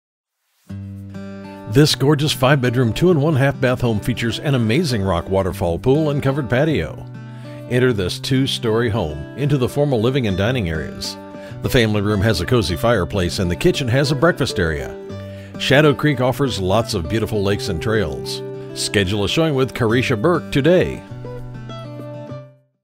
12628 Bethany Bay Drive Pearland, TX | ColdwellBankerHomes.com
12628 Bethany Bay Drive, Pearland, TX

Welcome to your new  home! close to the Pearland Town center with lots of shopping, restaurants and easy access to Hwy 288 , the Medical center or Downtown Houston. This gorgeous five bedroom, two and one half bath home nestled on a huge lot featuring an amazing  rock water fall pool and covered pa